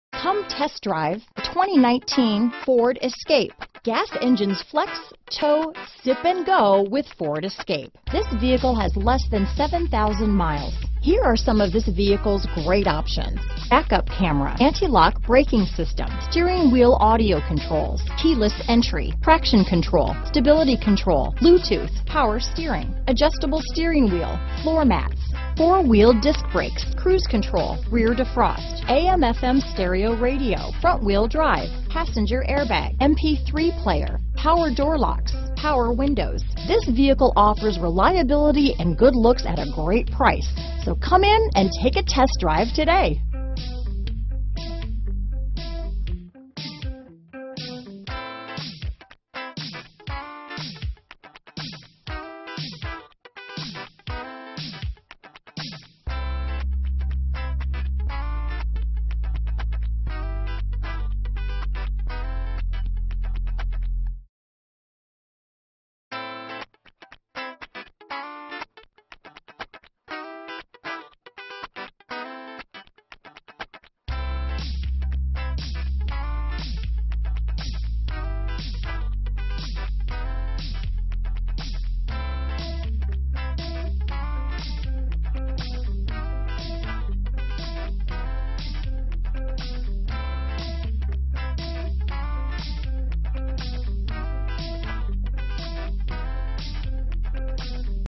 2019 Ford Escape Jacksonville, Swansboro, New Bern, Burgaw, Wilmington, NC T29666A
Silver Used 2019 Ford Escape available in Jacksonville, North Carolina at Sanders Ford. Servicing the Swansboro, New Bern, Burgaw, Wilmington, NC area.

Sanders Ford
1135 Lejeune Blvd.

Only 6,550 Miles! Boasts 29 Highway MPG and 21 City MPG! Carfax One-Owner Vehicle. This Ford Escape delivers a Regular Unleaded I-4 2.5 L/152 engine powering this Automatic transmission. Wheels: 17 Steel w/Sparkle Silver-Painted Covers, Variable Intermittent Wipers, Trip Computer.*This Ford Escape Comes Equipped with These Options *Transmission: 6-Speed Automatic w/SelectShift, Transmission w/Oil Cooler, Tires: P235/55R17 A/S BSW -inc: Low-Rolling-Resistance, mini spare, Tailgate/Rear Door Lock Included w/Power Door Locks, Systems Monitor, SYNC Communications & Entertainment System -inc: enhanced voice recognition communication, 911 Assist, 4.2 LCD screen in center stack, AppLink and 1 smart charging multimedia USB port, Strut Front Suspension w/Coil Springs, Steel Spare Wheel, Spare Tire Mounted Inside Under Cargo, Single Stainless Steel Exhaust.*Visit Us Today *Test drive this must-see, must-drive, must-own beauty today at Sanders Ford, 1135 Lejeune Blvd, Jacksonville, NC 28540.